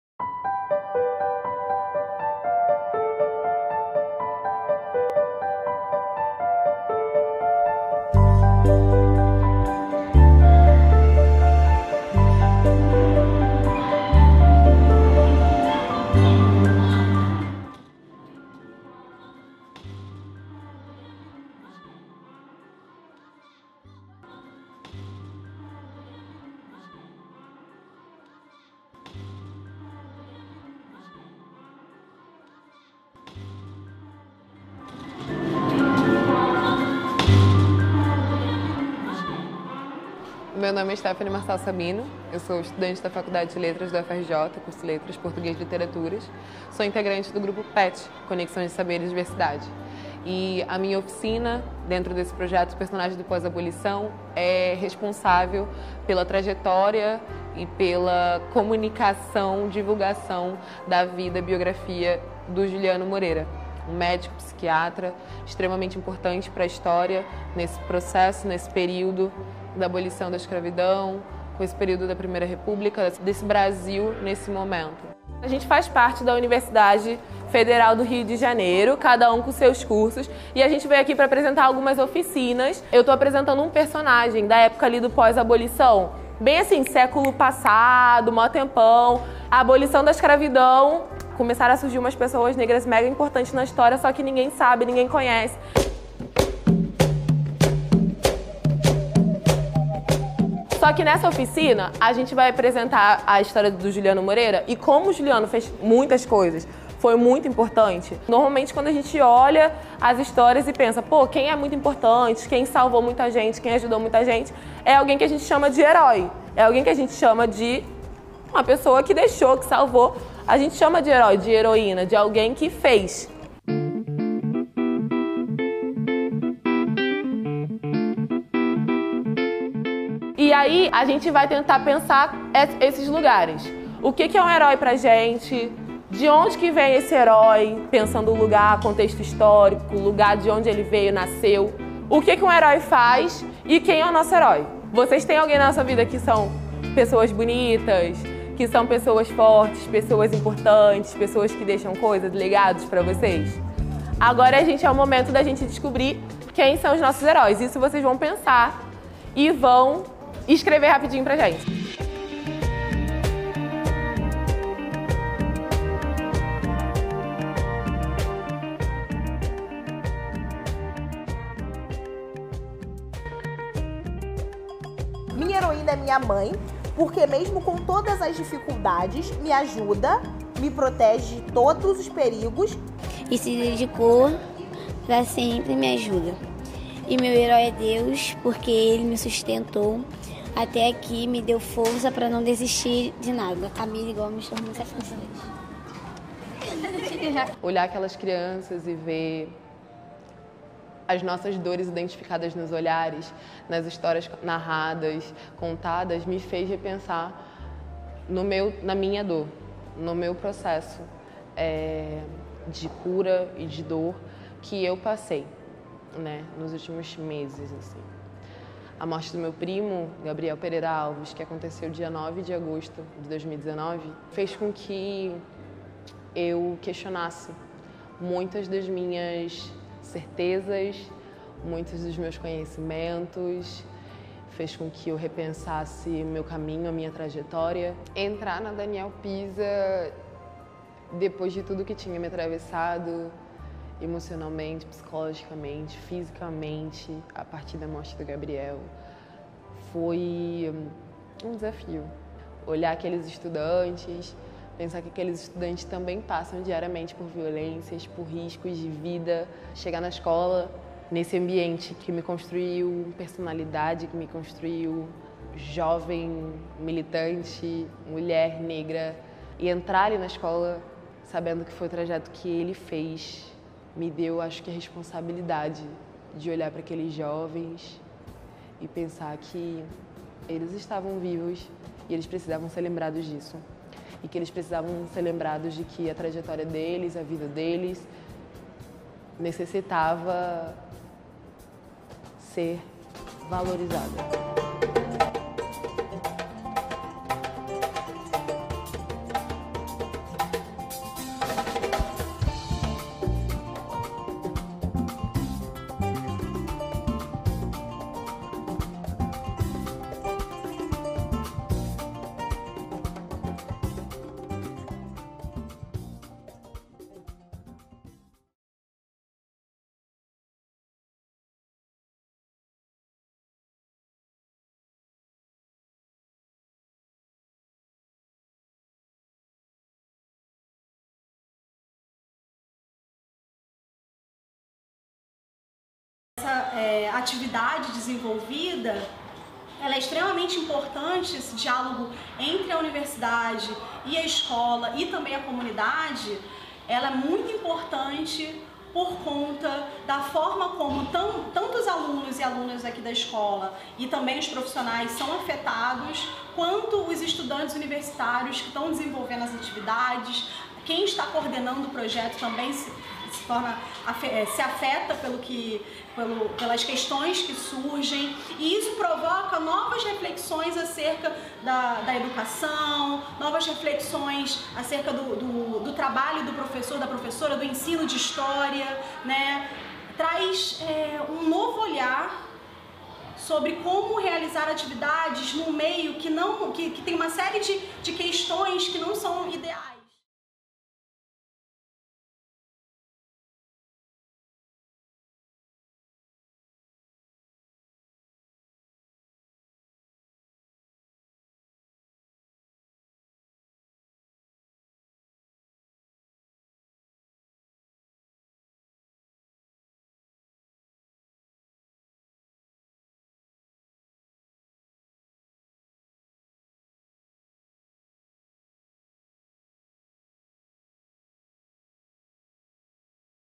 Pensamento Negro Acadêmico na UFRJ (2020) - Oficina Personagens do Pós-Abolição: Juliano Moreira
Vídeo de divulgação científica do Projeto Intelectuais Negr@s do Pós-Abolição (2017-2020), realizado na Escola Municipal Jornalista e Escritor Daniel Piza (RJ) pelo programa de Educação Tutorial Diversidade UFRJ. Coordenado pela profa. Dra. Giovana Xavier e ministrado pela graduanda Stéphane Marçal Sabino.

Projeto financiado pela Capes através do Edital Memórias Brasileiras: Biografias (no. 13/2015)